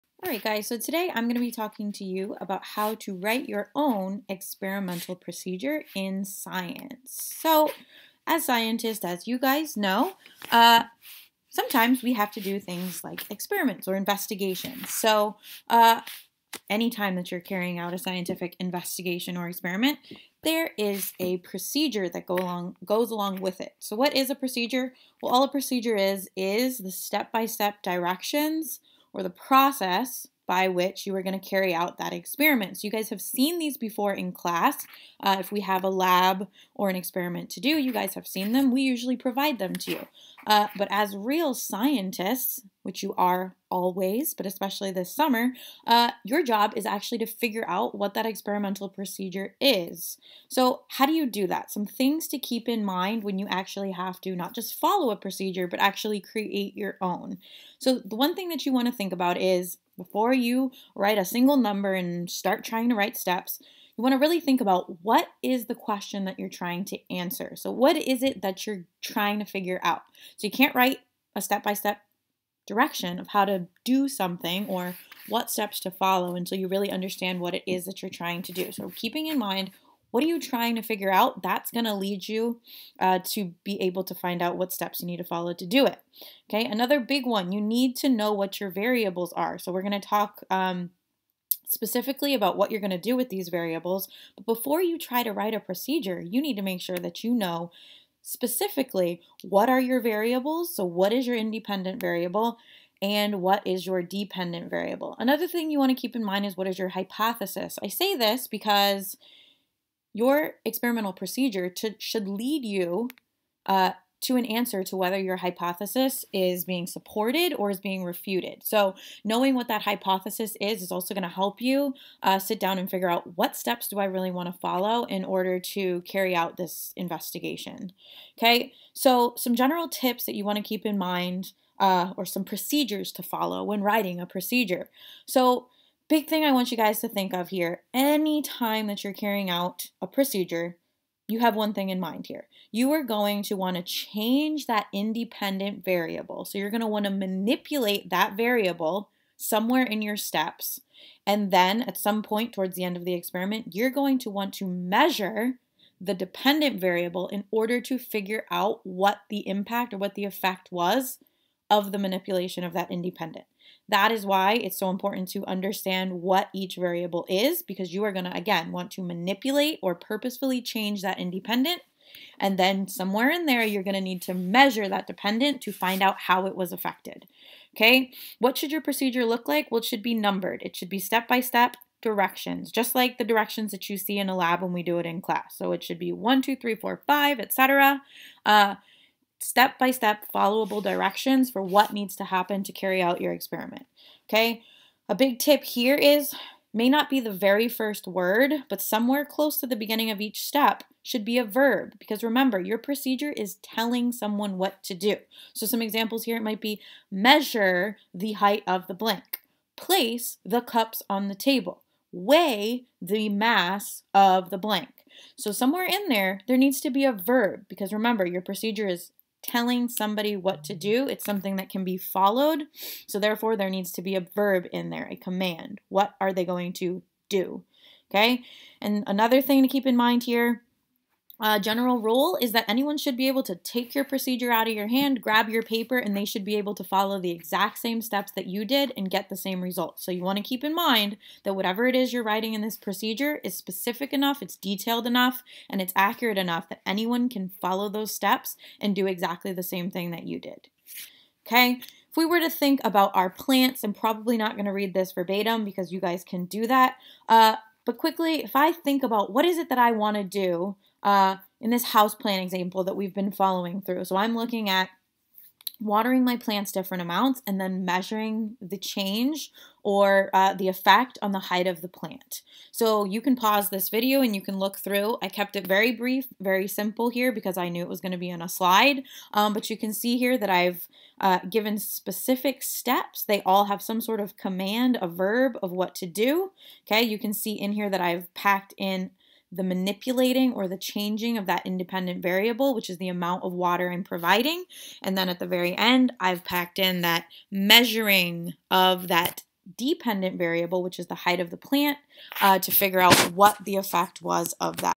Writing Experimental Procedures
How-To video for writing scientific procedures for students at the Crossroads school.